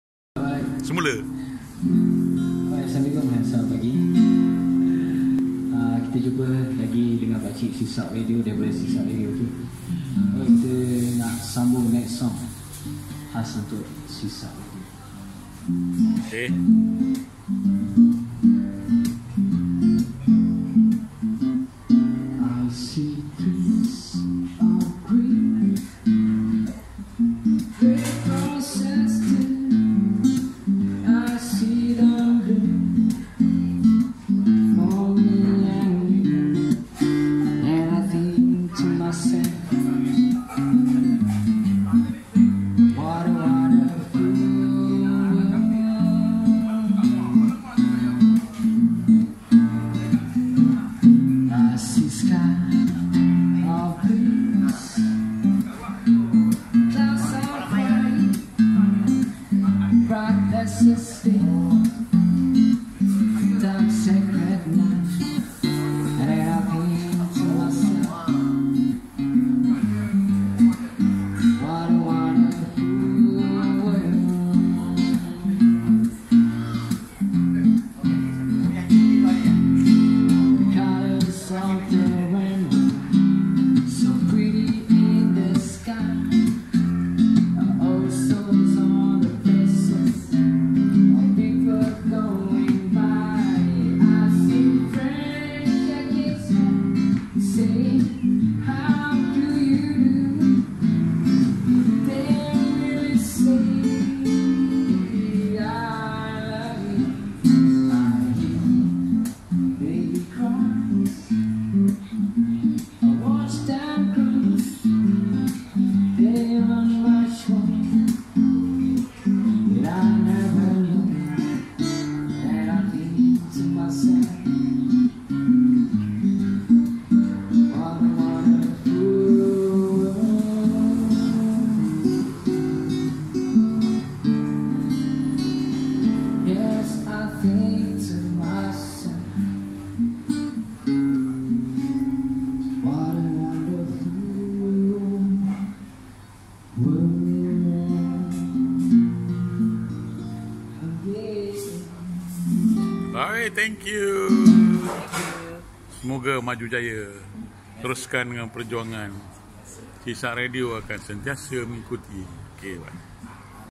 Busker Syaiful Anuar
Hebatnya Syaiful sebagai busker terkenal di Jaybee. Sysalq Radio kerap mengikuti perkembangan busker muda ini. Semoga terus berjaya.